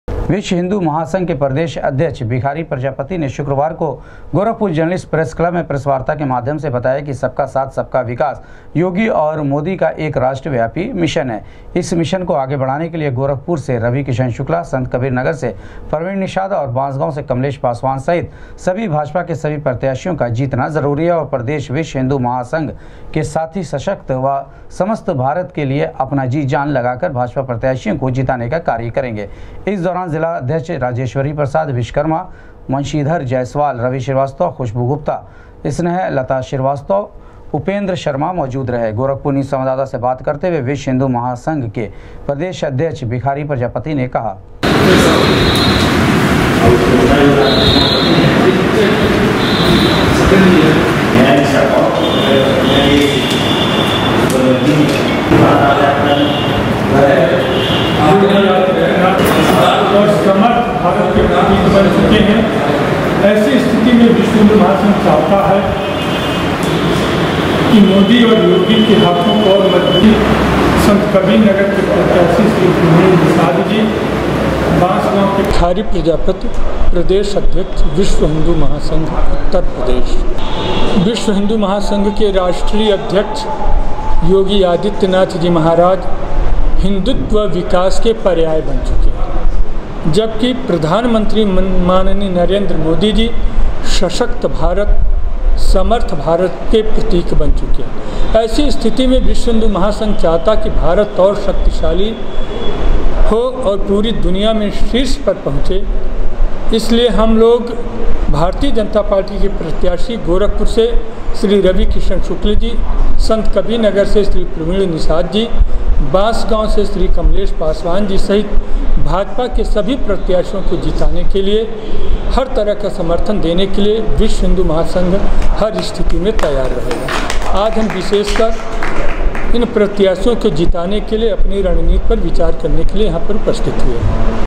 विश्व हिंदू महासंघ लोकसभा चुनाव में बीजेपी का किया समर्थन,विश्व हिंदू महासंघ के प्रदेश अध्यक्ष का
विश्व हिंदू महासंघ लोकसभा चुनाव में बीजेपी का किया समर्थन, विश्व हिंदू महासंघ के प्रदेश अध्यक्ष का कार्यकर्ताओं को निर्देश